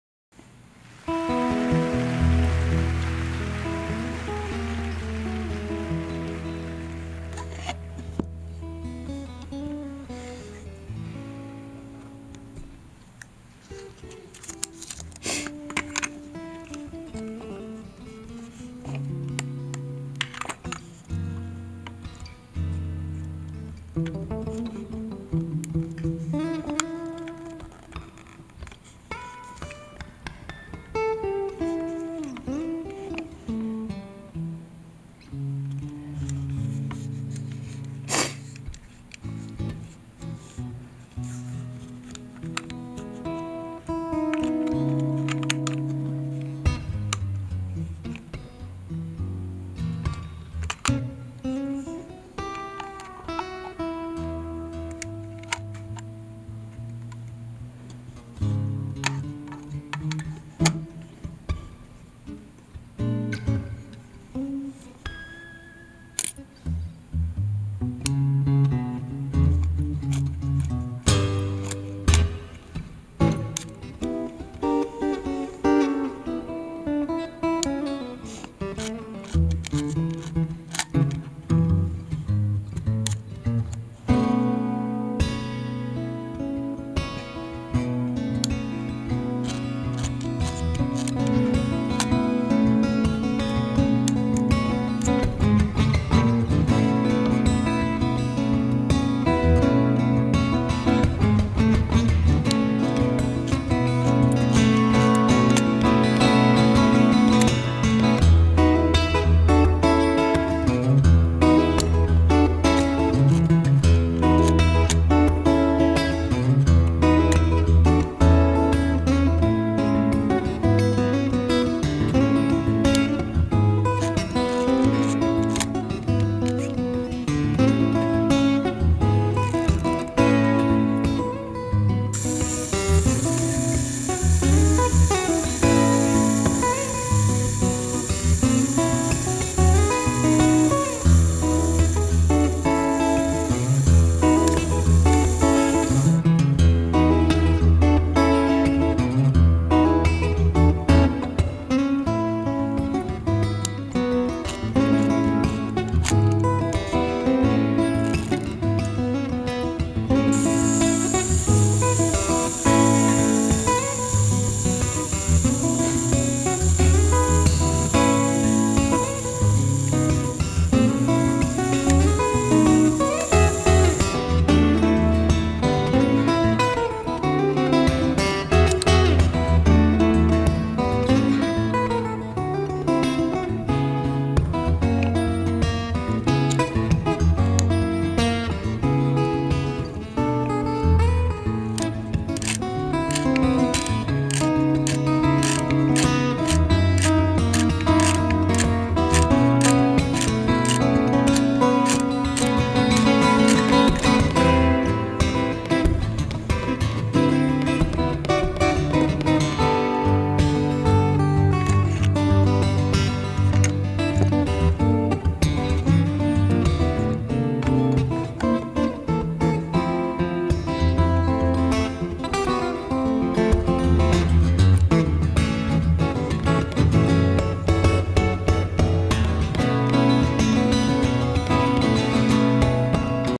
KODAK 35(f4 5)カメラの操作
1938～1948年にアメリカのイーストマンコダック社で初めて造りました35mmカメラです、ピント合わせは目測前玉回転式です。